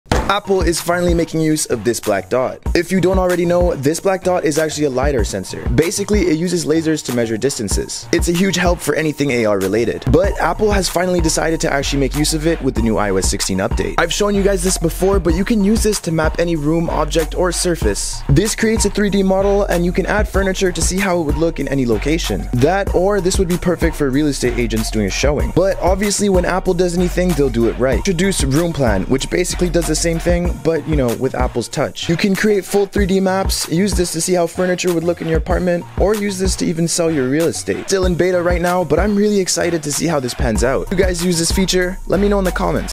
Apple Is Finally Using The Lidar Sensor...
Apple introduced roomplan which is going to be super useful to a a ton of people!